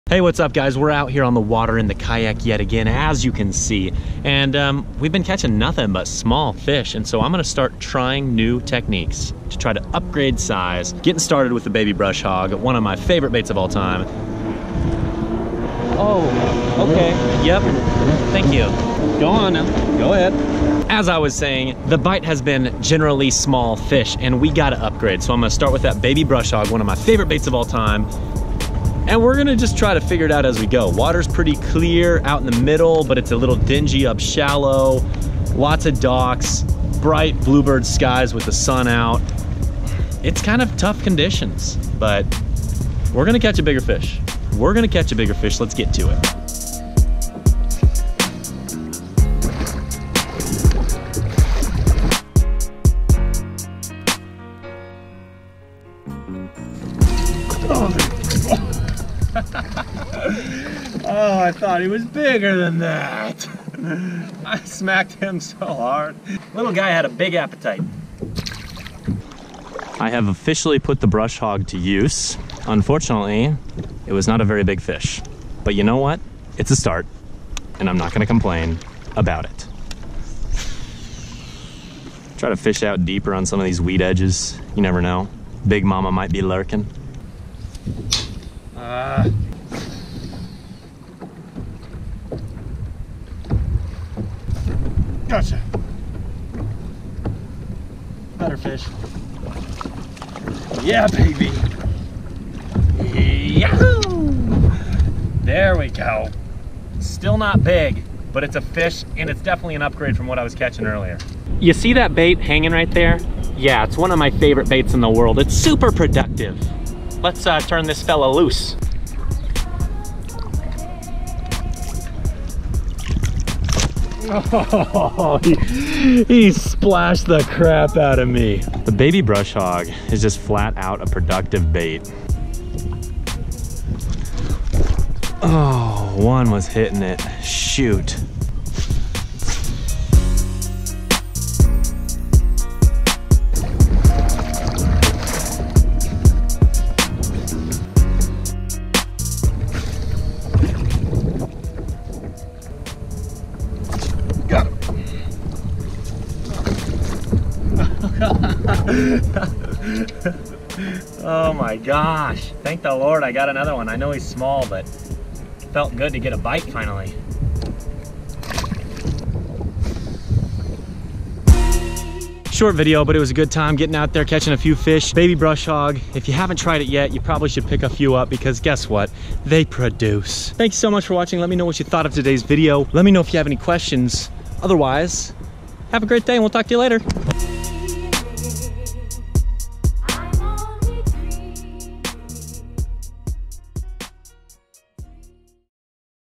Kayak Fishing with the Zoom Baby Brush Hog
The Zoom Baby Brush Hog is hands-down one of my favorite soft plastics of all time. When it comes to creature baits, I would argue that it might be the best out there. I like to fish it slowly through vegetation and brush piles on a 4/0 hook and light bullet weight.

On this particular day, I was kayak fishing and dragged it around offshore grass. The bite was very light so it was important that I paid close attention to my line.